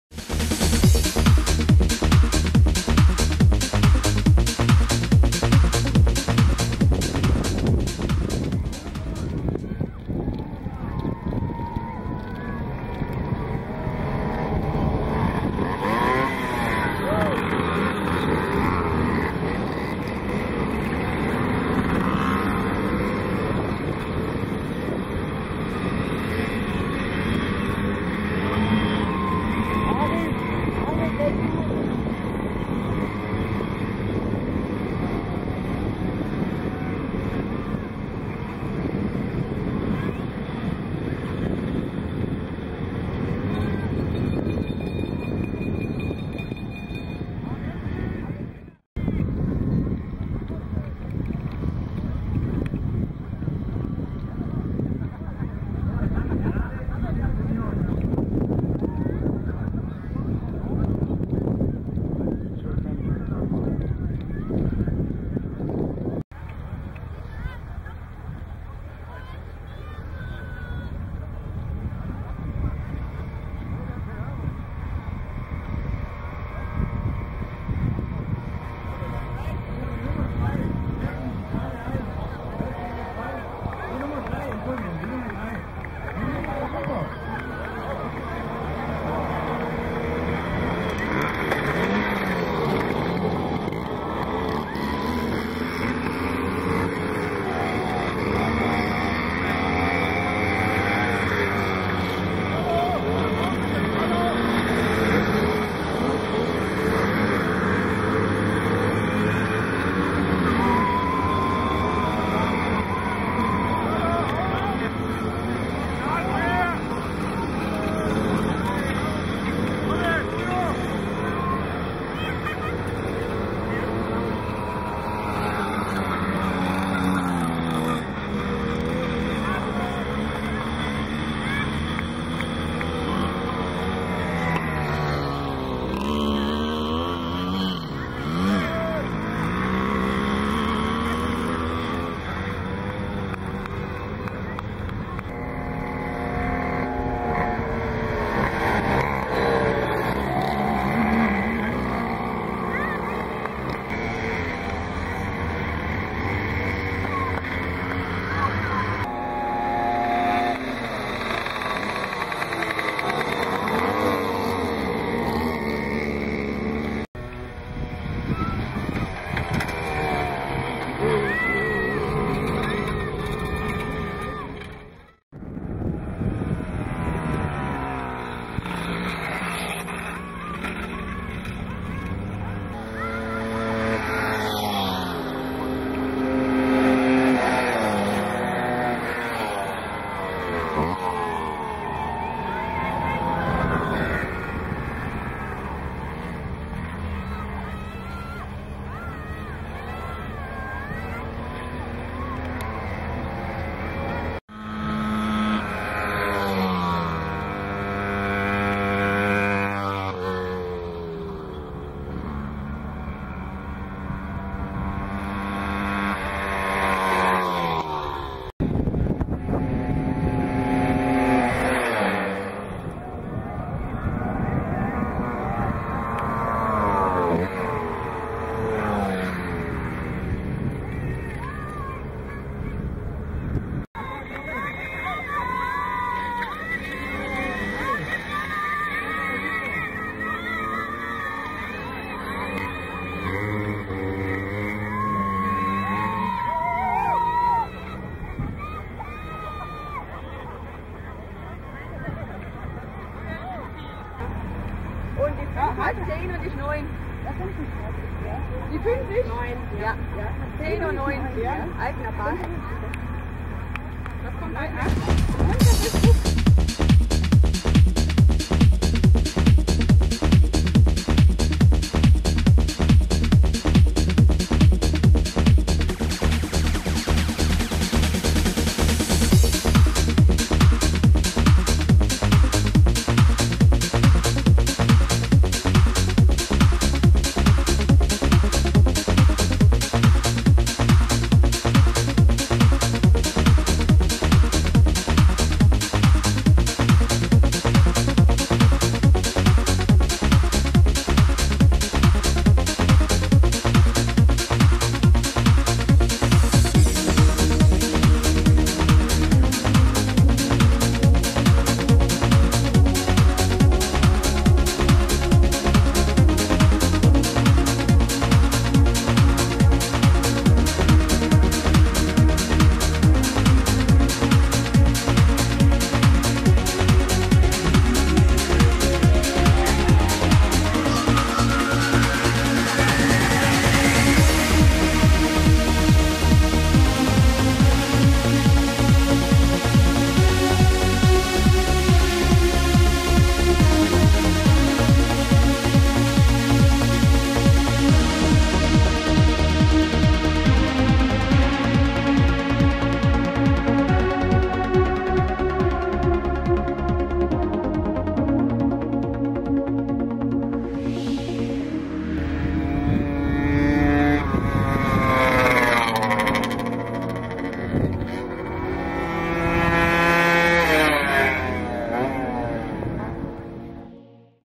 NSU Quickly Rennen 2009 Altheim/Staig "Race2"
NSU Quickly - Originale beim 20 jährigen Jubiläumsrennen in Altheim / Staig.